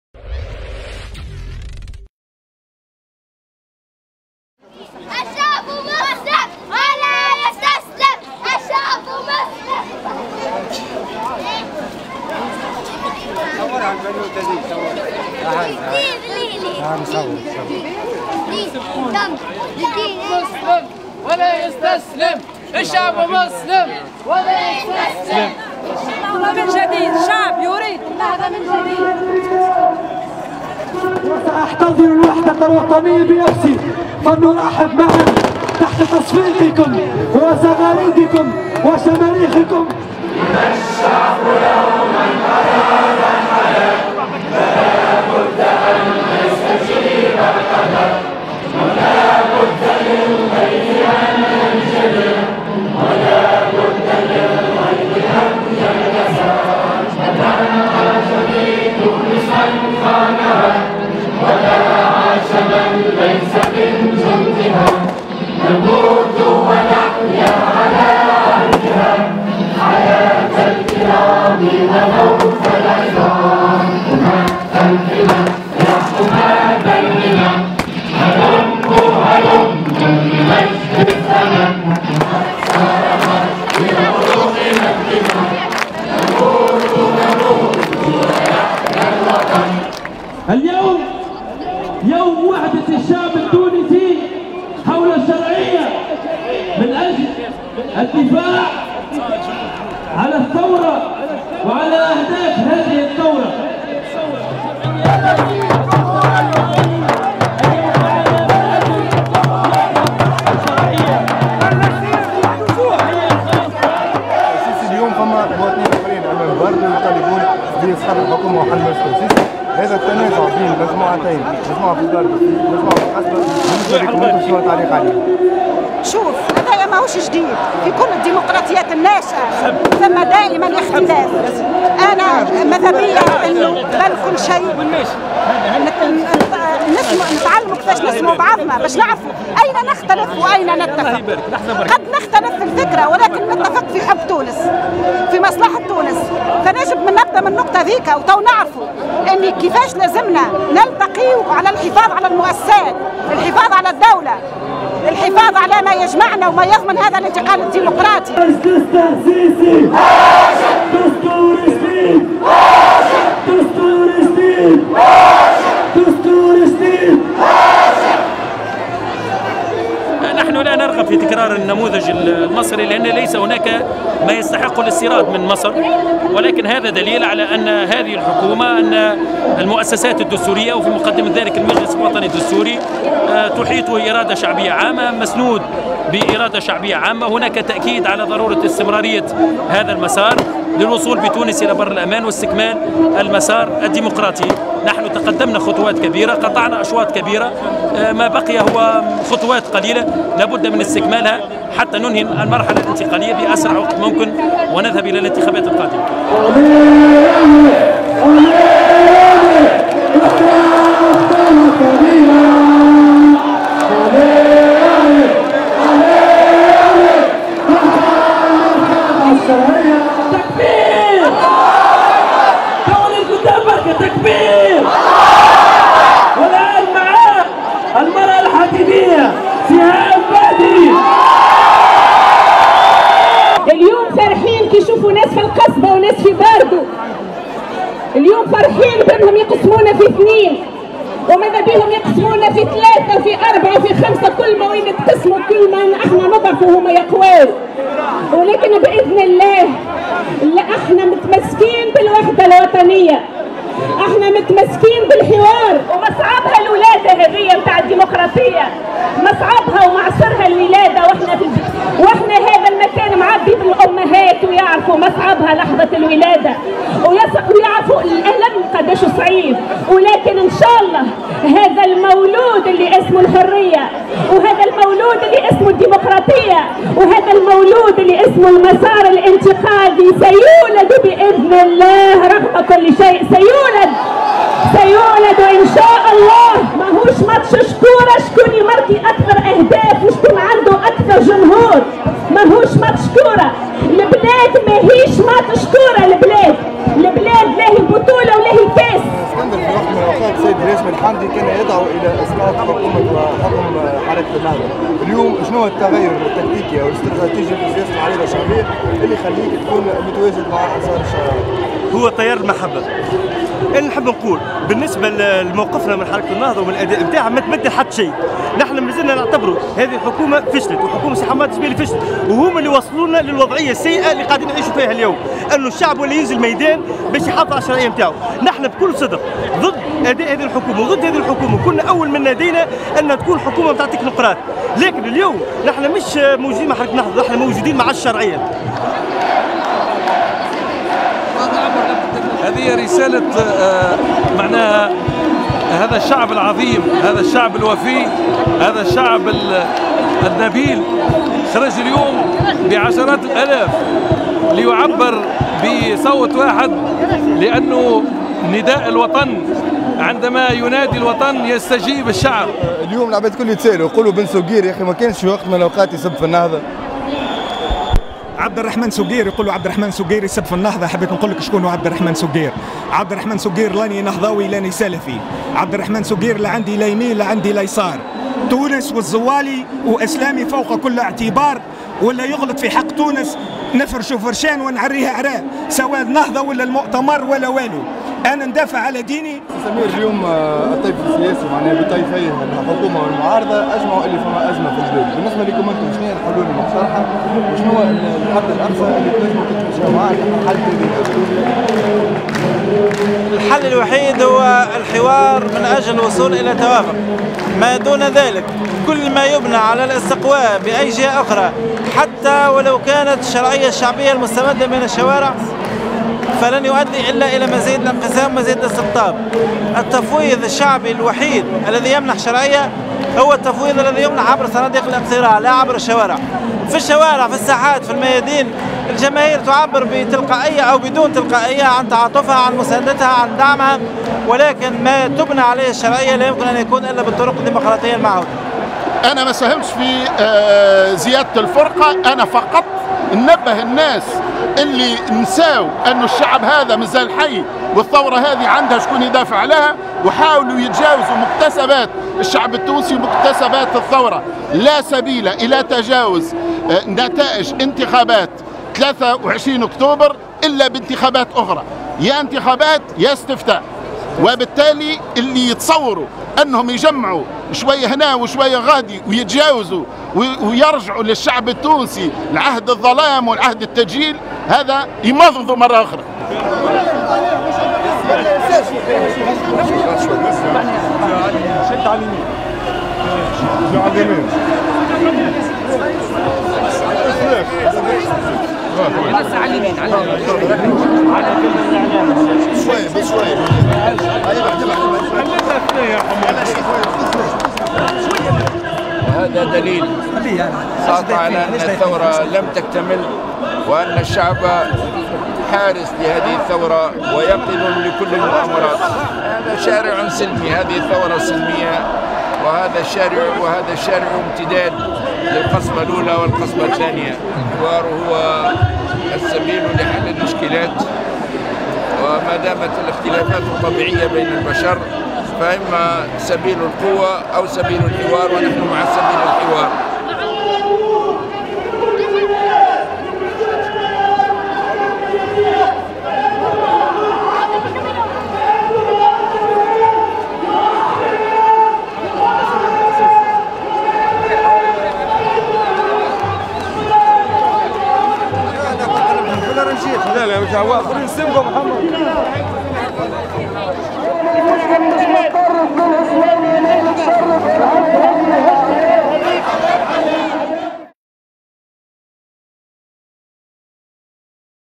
مليونية دعم الشرعية في القصبة : أجواء و حوارات
نظمت حركة النهضة اليوم 3 أوت انطلاقا من وقت الافطار ما أسمته ب"مليونية" دعم الشرعية في ساحة القصبة بالعاصمة .
و قد سخرت حركة النهضة و من نظموا معها هذه التظاهرة امكانيات هائلة و رصدت لتنظيمها مبالغ مالية ضخمة تظهر جليا في حجم مضخمات الصوت و نوعية الاضاءة .
الحضور كان غفيرا و لكنه لم يبلغ المليون كي تكون مليونية و اقتصر على الاف عديدة قدمت من كل حدب و صوب و من كل ولايات الجمهورية و كذلك من احياء و مناطق عديدة بالعاصمة .
كنا على عين المكان و رصدنا لكم اجواء هذه التظاهرة و قمنا بحوارات مع وجوه سياسية بارزة من حركة النهضة
و أعددنا لكم الروبرتاج التالي :
متابعة و تصوير و مونتاج : أمين مطيراوي